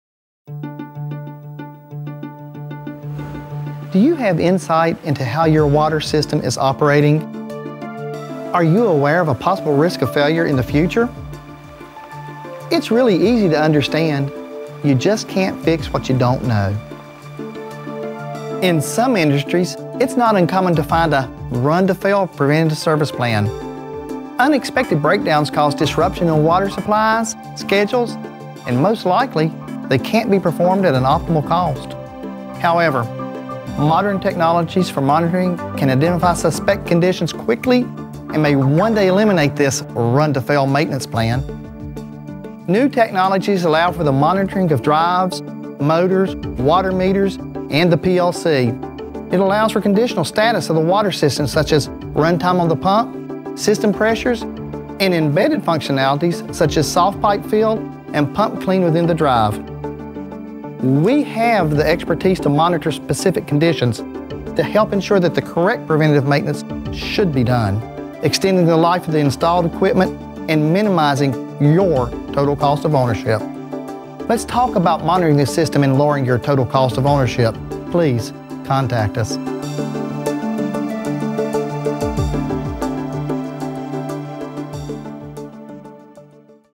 ABB Water Expert – Remote monitoring
ABB’s water expert talks about how ABB can improve utility operation with ABB’s remote condition monitoring solutions for water and wastewater.